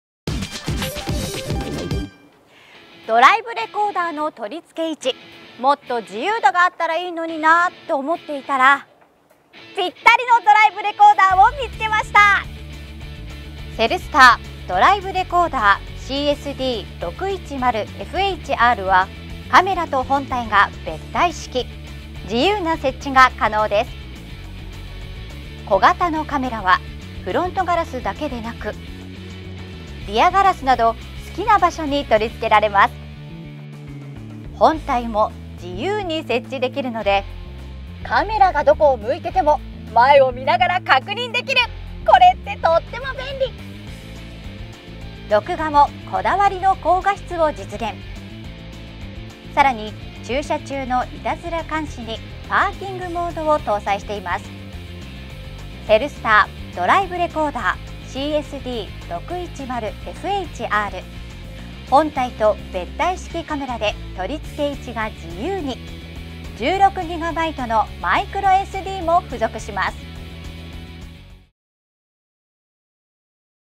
【カーグッズフォーカス】セルスター工業 「ドライブレコーダーCSD-610FHR」
カー用品が動画で見られる『カーグッズフォーカス』をチェック！！

セルスター工業　「ドライブレコーダーCSD-610FHR」

画像が美しく後方撮影も可能

200万画素のカメラと2.4インチの液晶モニターが分離しているドライブレコーダー。9mのカメラ接続コードが付属しており、カメラはリヤガラスなどにも取り付け可能。HDR搭載により、白飛びや黒つぶれも低減されている。